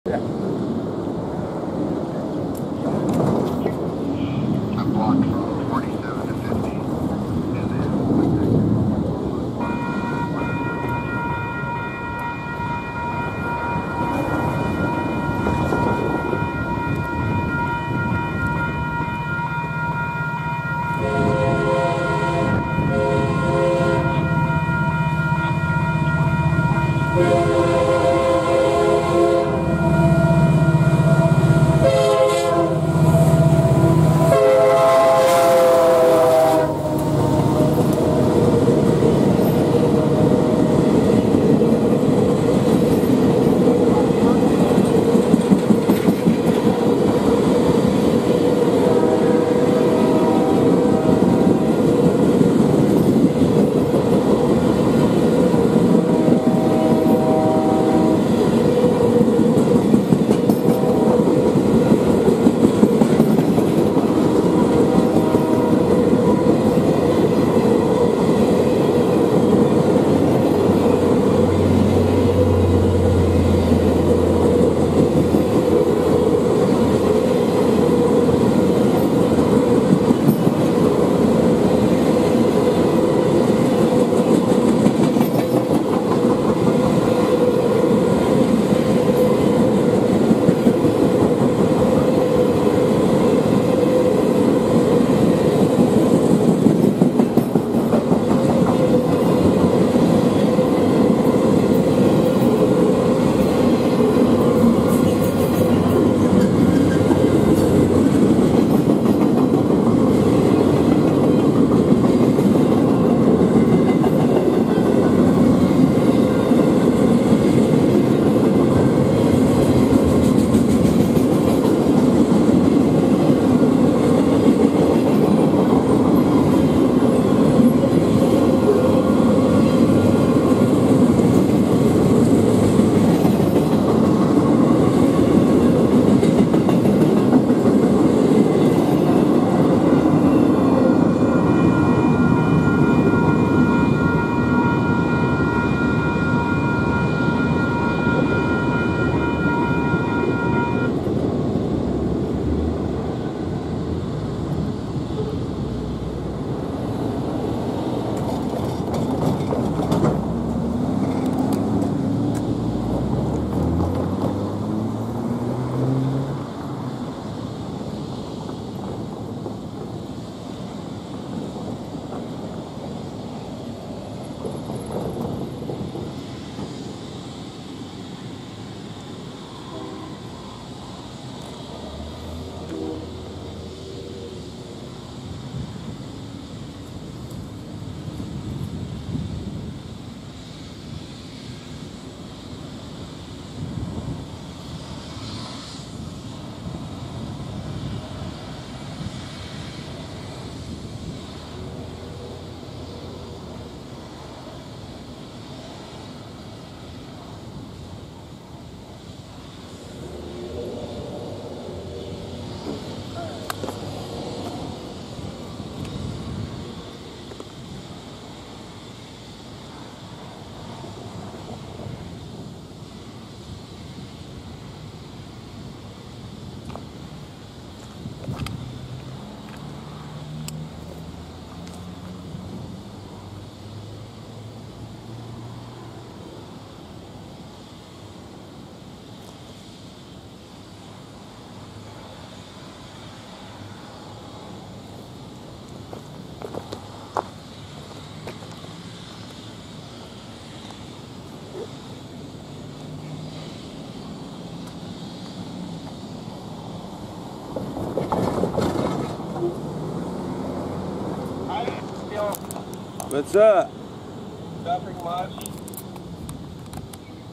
Ringling Brothers Red Circus train leaves Ontario for Anahiem
This was a few weeks ago, i had missed it coming into Ontario the day before due to me being in Ely, NV. But i was able to see it leave Ontario for Anahiem. It was also my first time seeing a UP loco quill its horn. Hope you enjoy this video